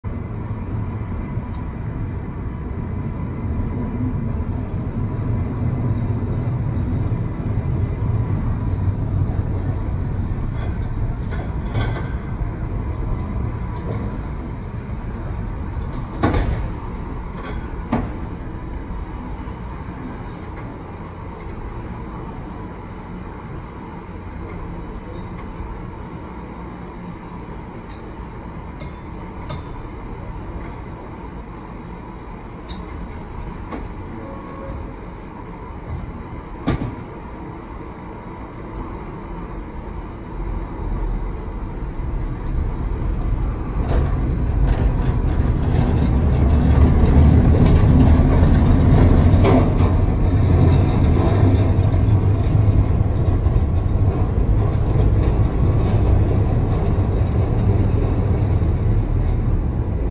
Powell Hyde Cable Car 9 @ Jackson St & Taylor St San Francisco California (Slow Motion)
Powell Hyde Cable Car 9 @ Jackson St & Taylor St San Francisco California

4K 30fps

December 2023